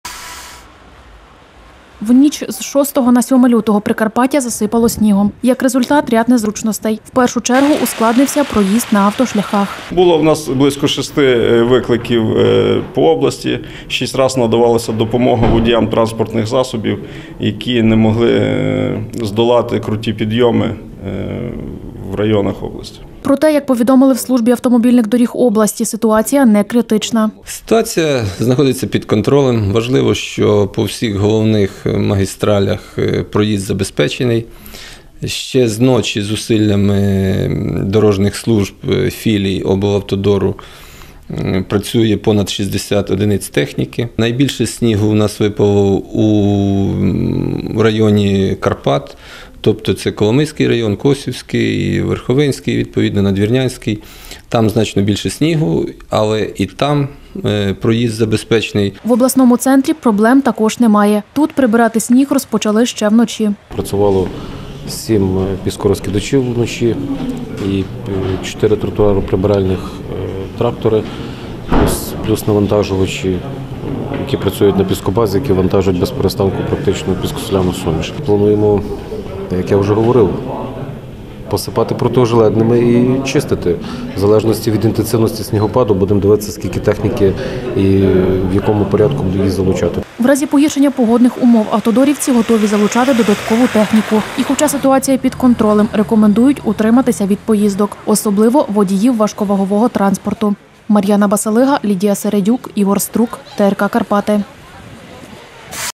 Прикарпаття засипало снігом
В ніч з 6 на 7 лютого Прикарпаття засипало снігом. Як результат – ряд незручностей. В першу чергу ускладнився проїзд на автошляхах.
В разі погіршення погодних умов автодорівці готові залучати додаткову техніку. І хоча ситуація під контролем, рекомендують утриматись від поїздок, особливо водіїв важковагового транспорту.